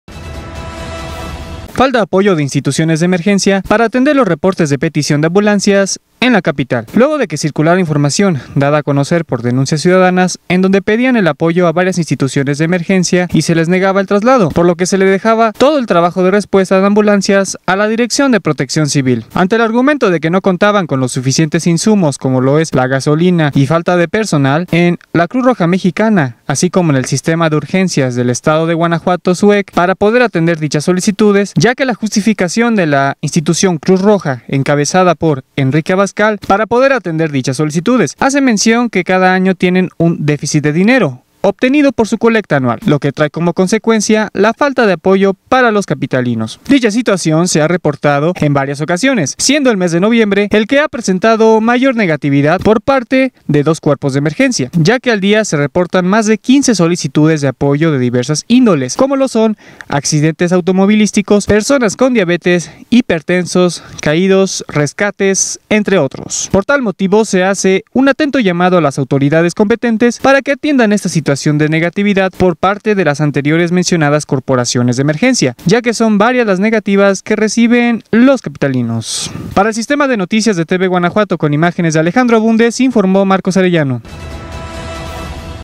Atiende PC las emergencias debido a negligencia de Cruz Roja y SUEG
La negligencia de la CruzRoja y de la SUEG para atender con sus ambulancias las emergencias en Guanajuato capital, han terminado por cargarle la chamba a protección civil